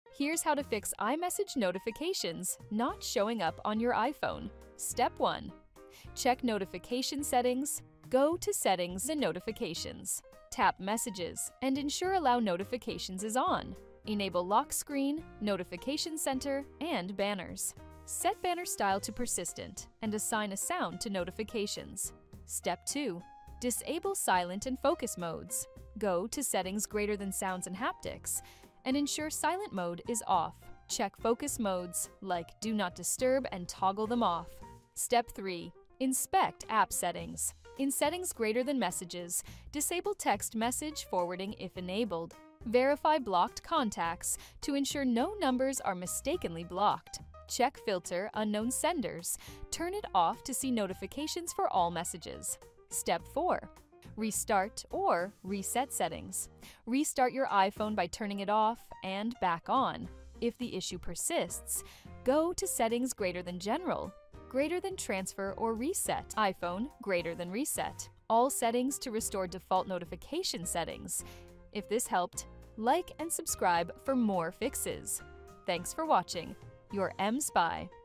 How To Fix iMessage Notifications Not Showing Up On iPhone
Trouble with iMessage notifications? Follow steps on the video to get them working again.